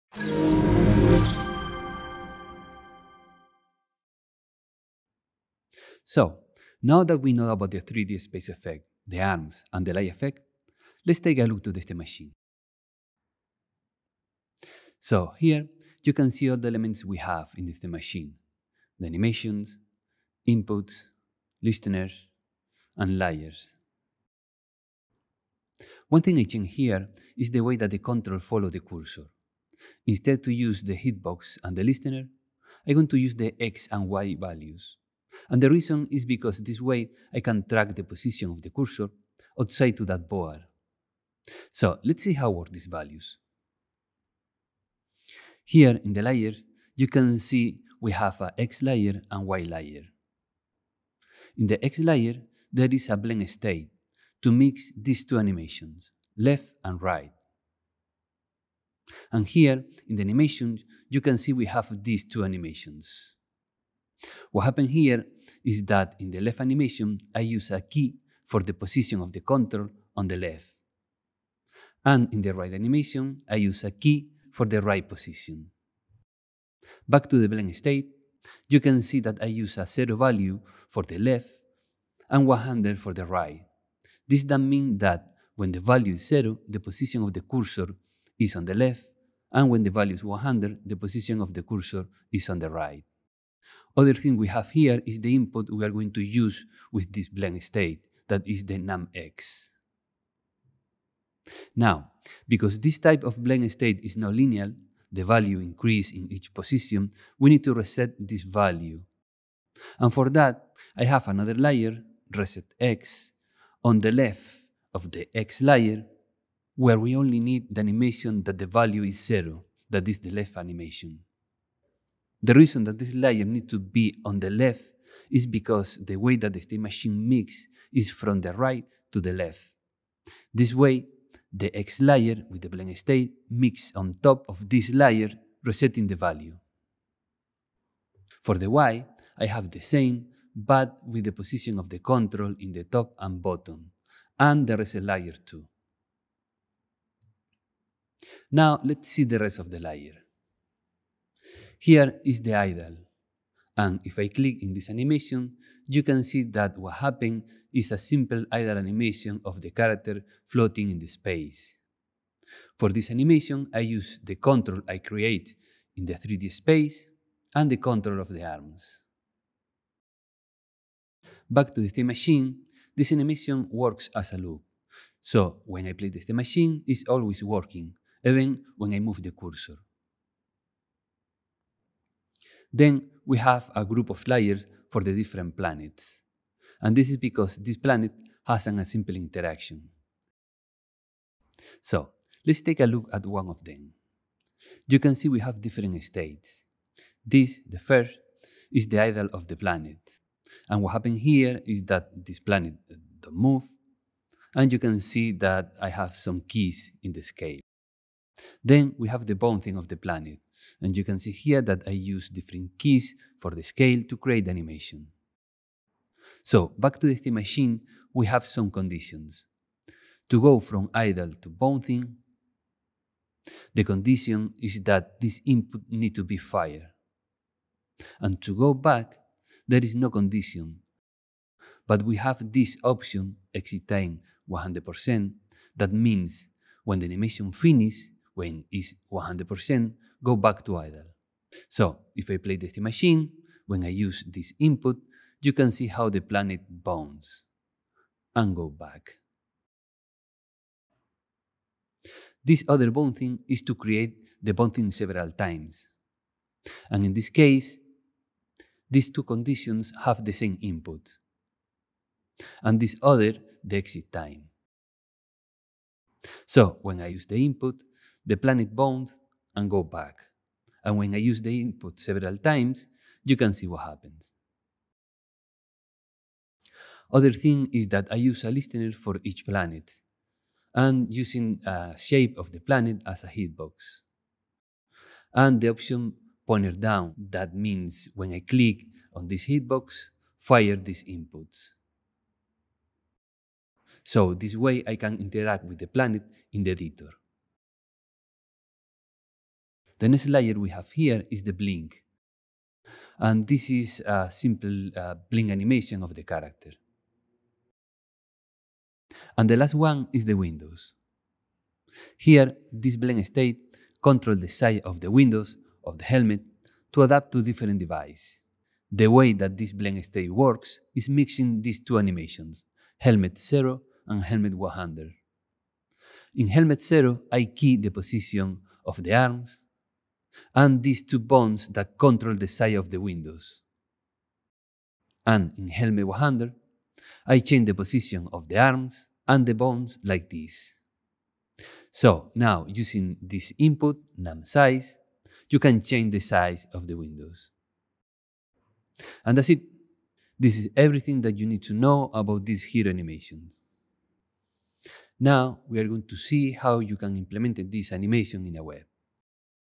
Learn how the state machine works for this hero animation
We look at how the state machine works for this hero animation and how to make the control follow the cursor using X and Y values.

This video is part of a tutorial series on creating interactive hero animations.